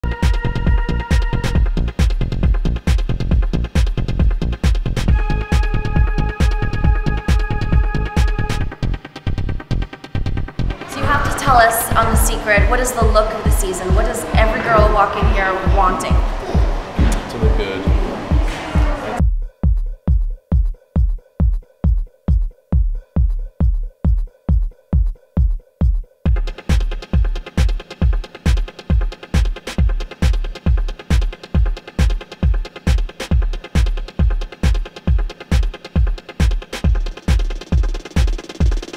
FASHION & STYLE at Ion Studio
WATCH: FASHION & STYLE co-host Natascha Bessez recently had hair touched up by hair stylist and co-owner Marco Santini of Ion Studio. In this video Natascha asks Mack about the hottest hairstyles this season which hairdos are the most requested at the salon. Gorgeous Natascha is an actress, model, singer and former "Miss New York Teen USA".